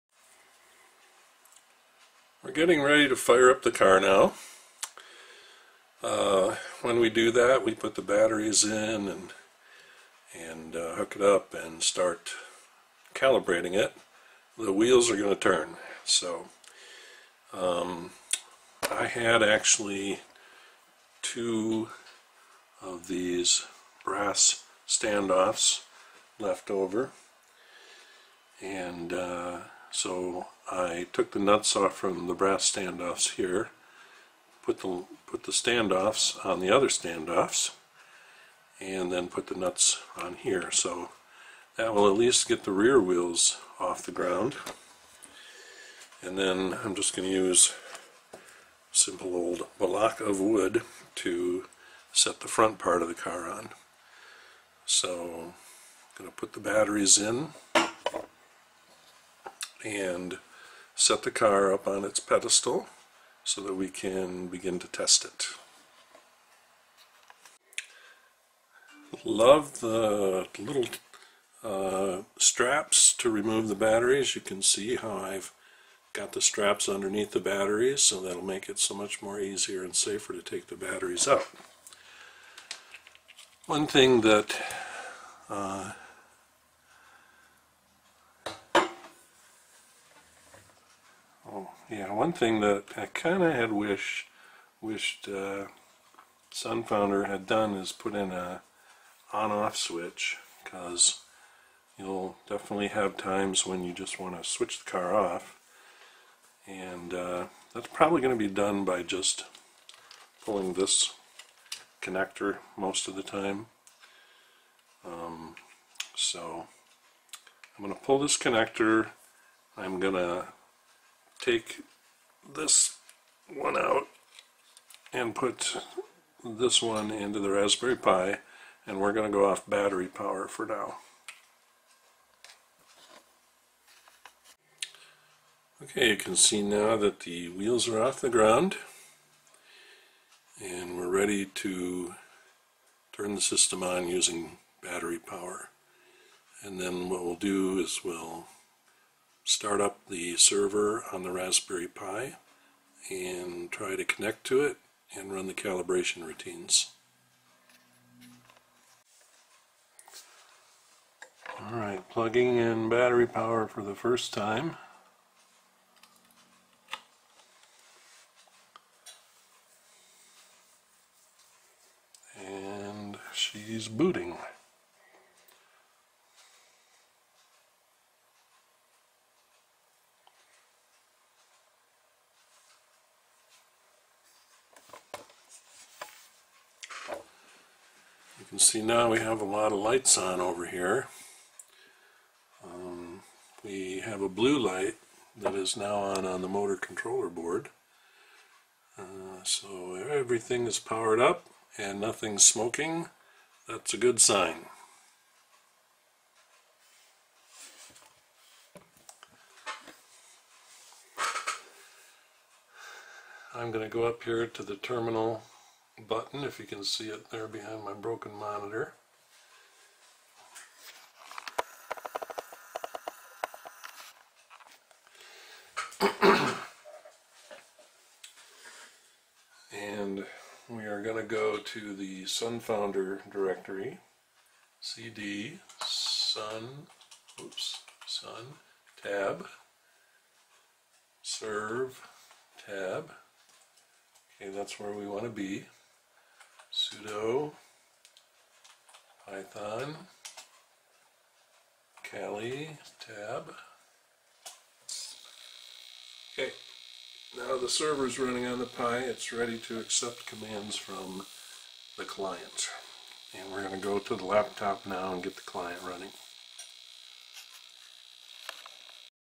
Sunfounder Video Car 33 first power up of car on batteries
Putting the car up on blocks and testing for smoke. Starting the server on the car under battery power.